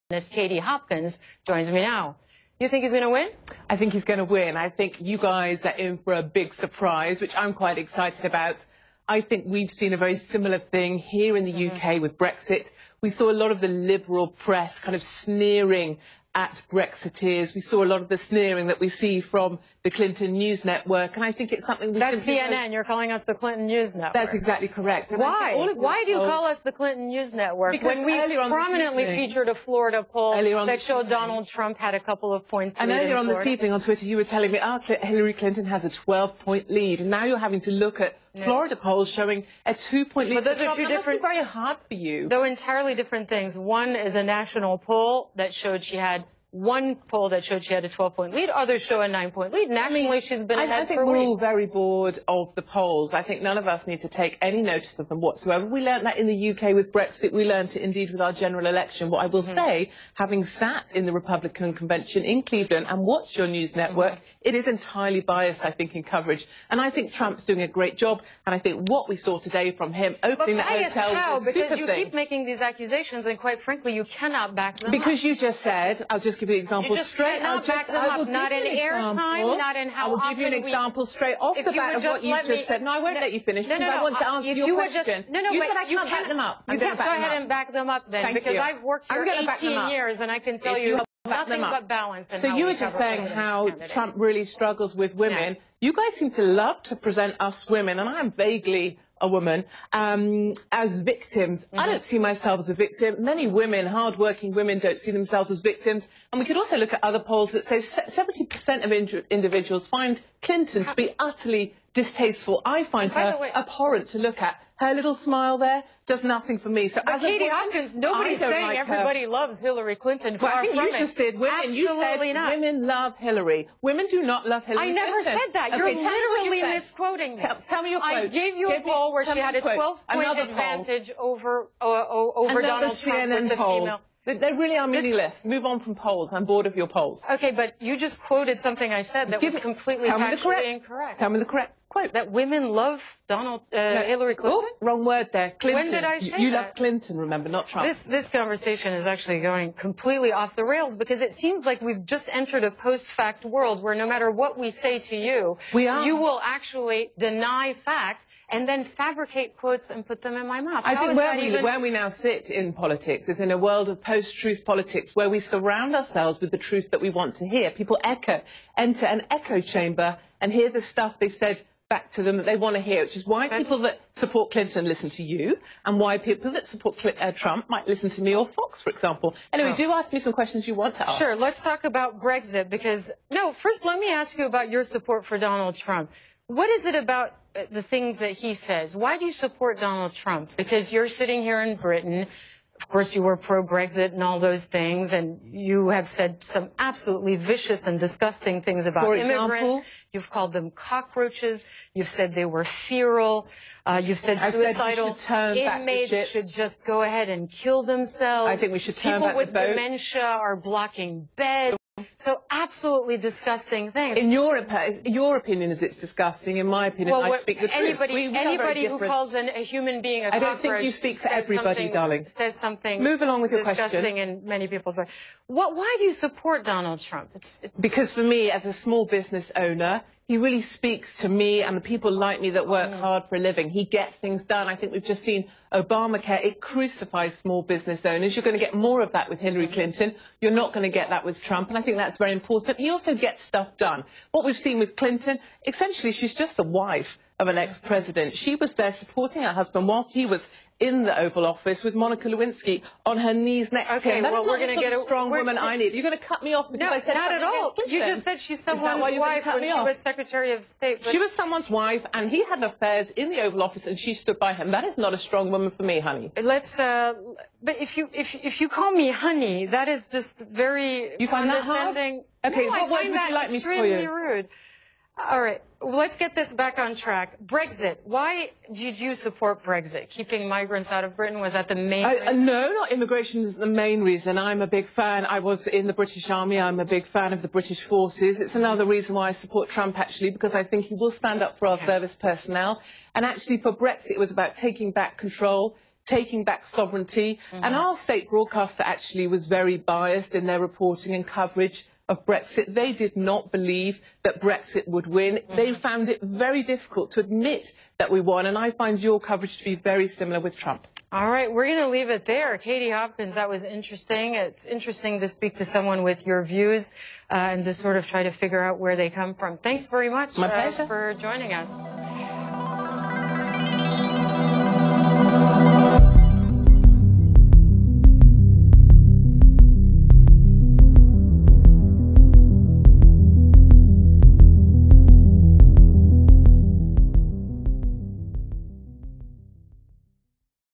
CNN the Clinton News Network, Liberal Anchor loses It On Air!
Recently an outspoken British reporter for the U.K. Daily Mail made an appearance on CNN International in London and put into words, while live and on the air, what millions of Americans have been saying to themselves and others throughout the 2016 election cycle, if not for years before that.

Journalist Katie Hopkins called out CNN for being the “Clinton News Network,” citing its overwhelming bias in favor of Democrat presidential nominee Hillary Clinton, and refused to back down from her assertion when challenged by the stammeringly self-righteous and indignant host, Hala Gorani.

Asked about her support for Republican nominee Donald Trump, Hopkins proclaimed, “I think he’s going to win. I think you guys are in for a big surprise which I am quite excited about.”